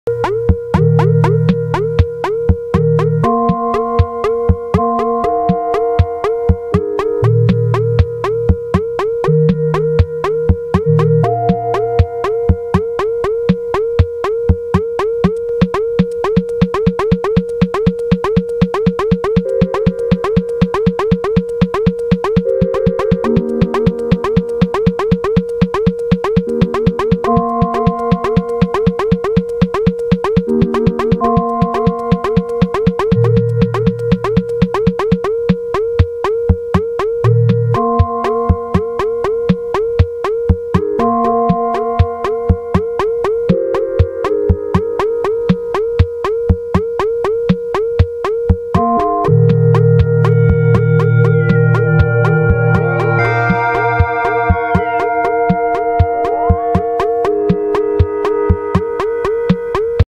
Moog DFAM, Arturia Microfreak voocoder, volca beats, Ipad Mononoke Jam
Arturia Microfreak vocoder ed. +
Moog Dfam +
Volca Beats +
Volca Mix
Mononoke App

Blossom Bisquits
Electronica IDM & Dark Ambient
Musician from Ravenna / Italy
Guitar/Synth/Samples/Drum machine